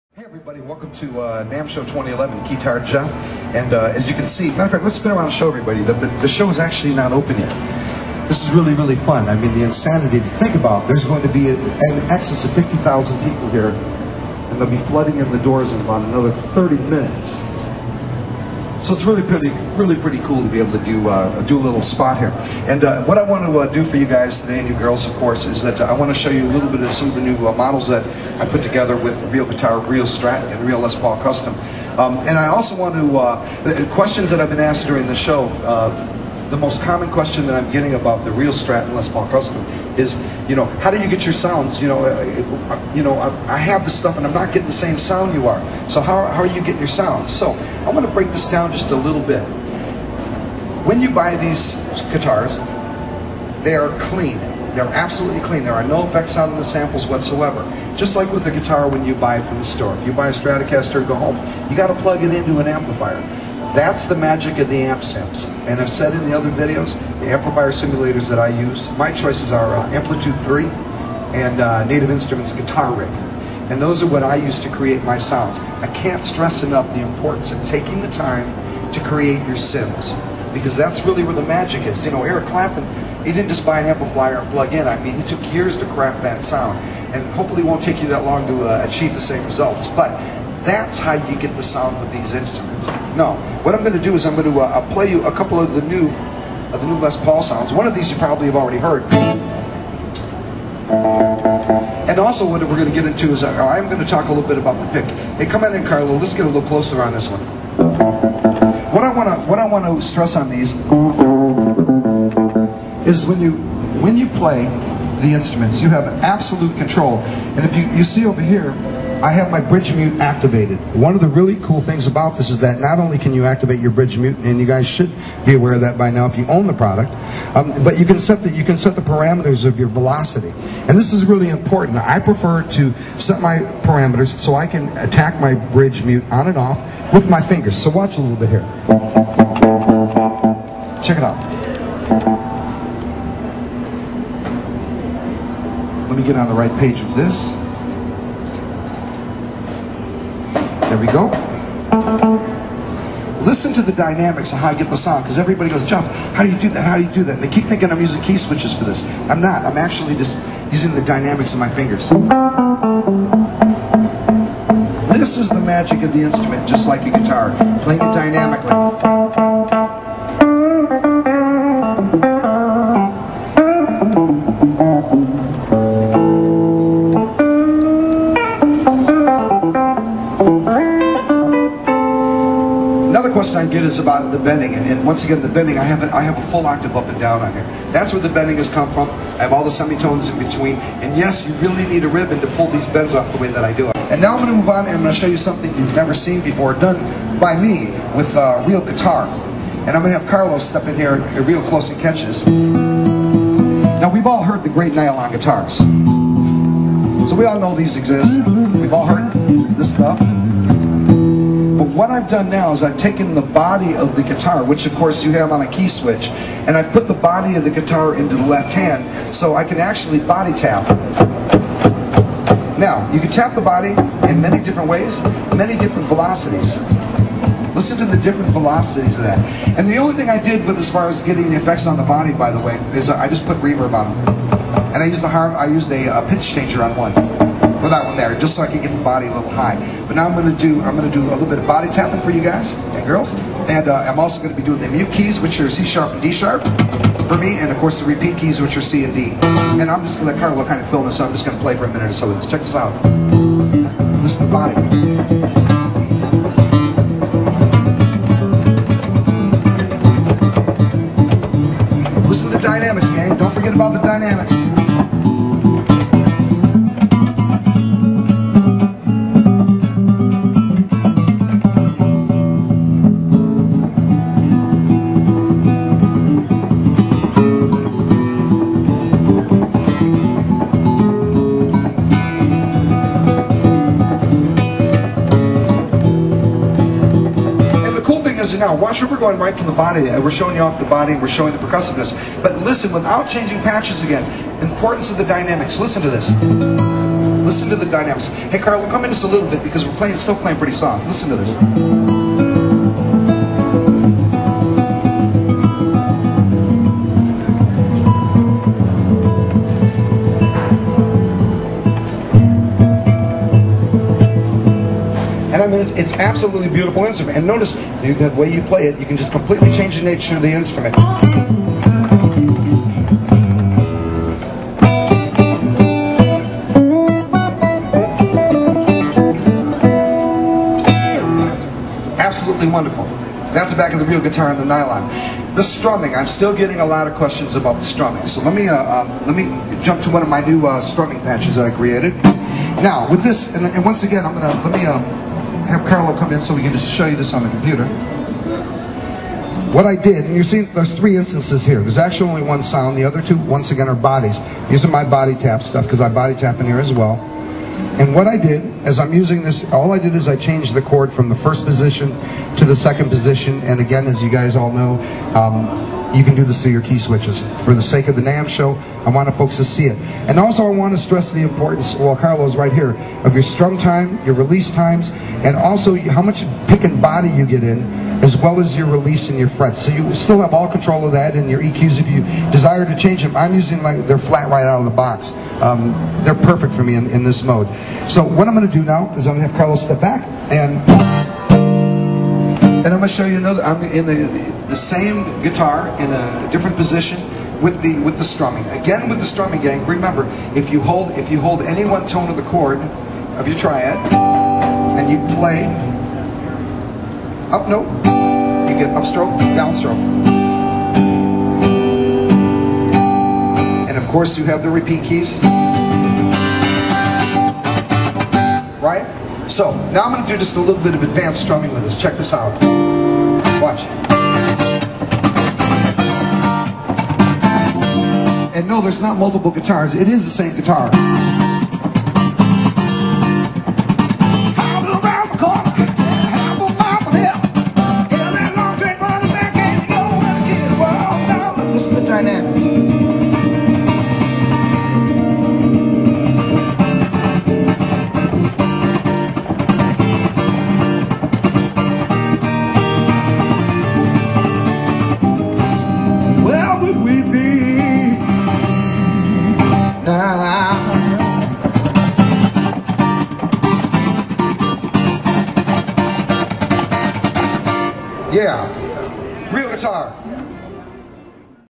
Keytarjeff NAMM 2011 RealGuitar(s) Tips(part 1).mp4
Keytarjeff explains Amplifier Simulators,as well as Strumming and Picking techniques with RealStrat, Real LPC&RealGuitar from MusicLab.